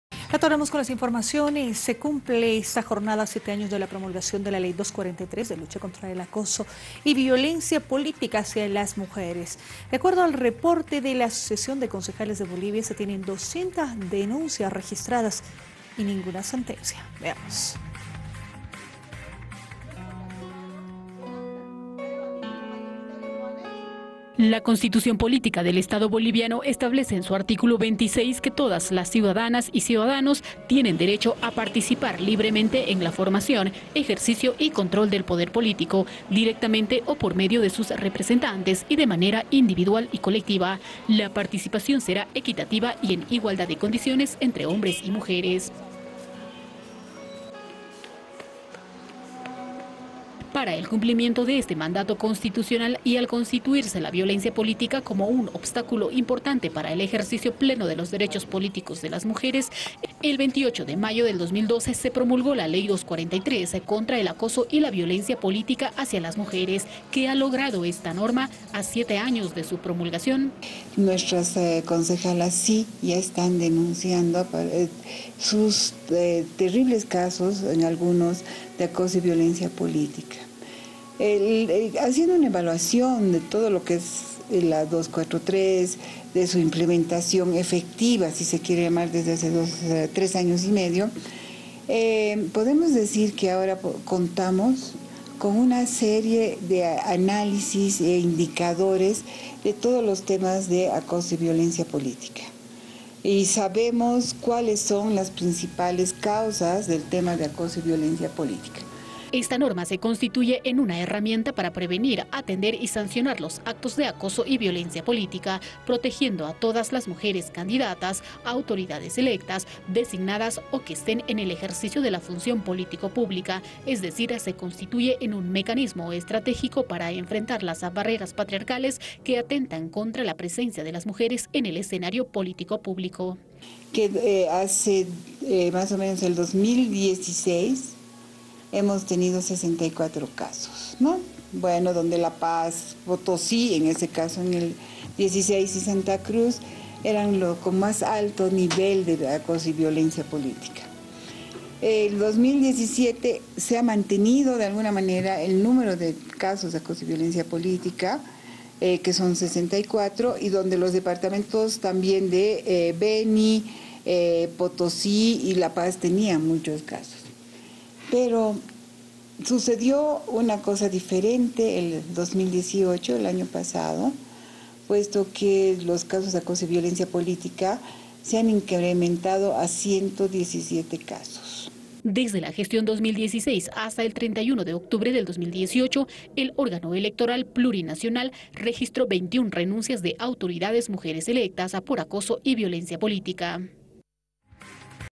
En siete años no se emitió una sentencia por acoso y violencia política en Bolivia
A siete años de la promulgación de la Ley 243 Contra el Acoso y Violencia Política hacia las Mujeres, se presentaron más de 200 denuncias por este delito ante distintas instituciones, de las cuales ninguna recibió una sentencia.